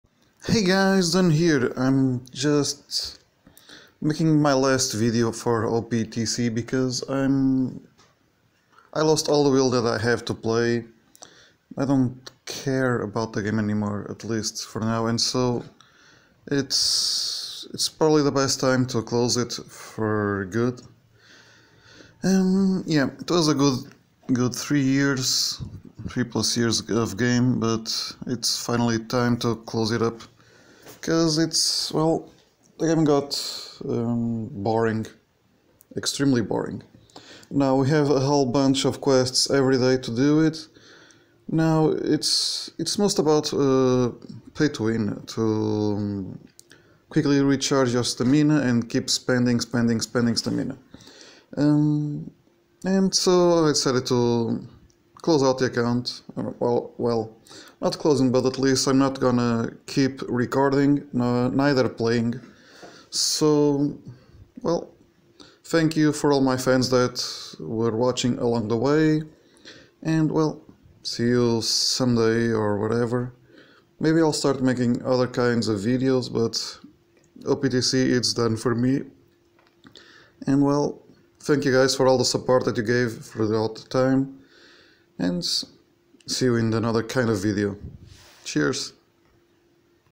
End of OPTC for me and recordings
Thank you all for your support but its time to move on.

Was fun while it lasted.